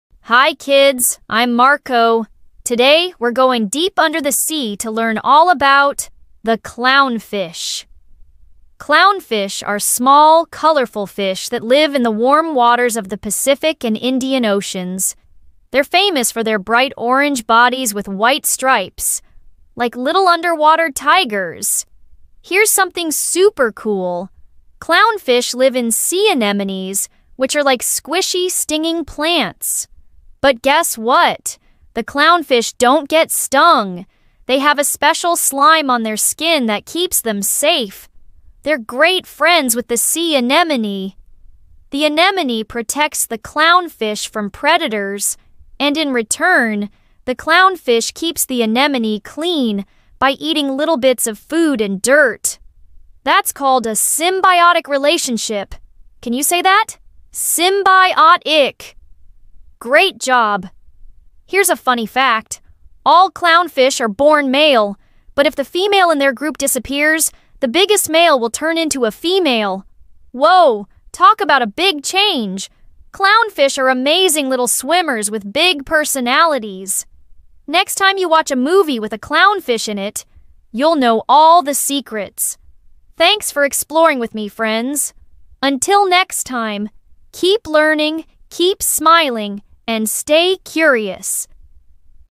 Clown Fish for Kids | Amazing Fun Facts and Information | Learn with Marko
Hi Kids! I’m Marko, and today we’re diving deep into the ocean to learn all about one of the ocean’s most colorful and clever fish — the clownfish! 🧡🐟

In this exciting 1.5-minute adventure, you'll discover:
✅ Why clownfish live in sea anemones
✅ How they protect each other
✅ A funny fact about clownfish changing from male to female!
✅ And what symbiotic really means!

Whether you love fish, the ocean, or just learning cool new things — this video is just for YOU!

💡 Perfect for preschoolers, kindergarteners, and curious young explorers everywhere.